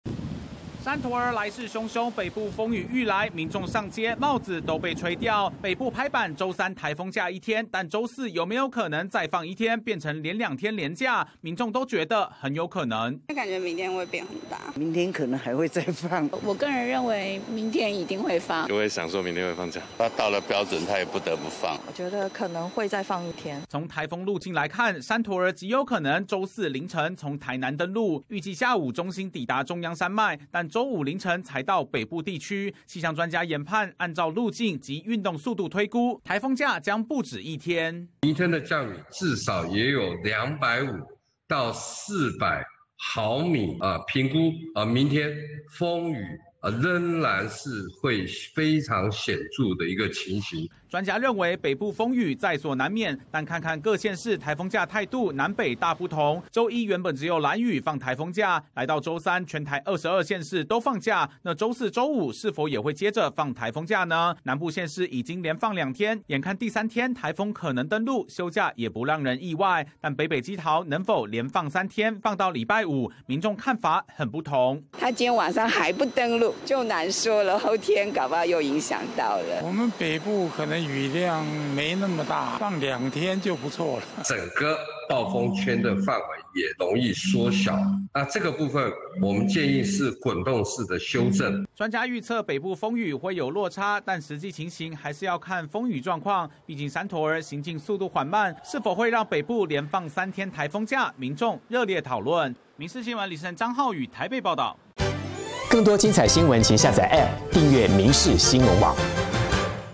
北部將放3天颱風假？山陀兒超級龜速「估週四清晨」台南登陸－民視新聞
【民視即時新聞】生活中心／李世宸、張皓宇　台北報導山陀兒來襲，全台22縣市，今天都放颱風假，南台灣更是邁入第二天，只是颱風龜速前進，氣象專家根據資訊，大膽預估颱風假極有可能「不只一天」，南部風雨甚至可能放到第三天，北部也可能連放3天嗎？一起來看看！。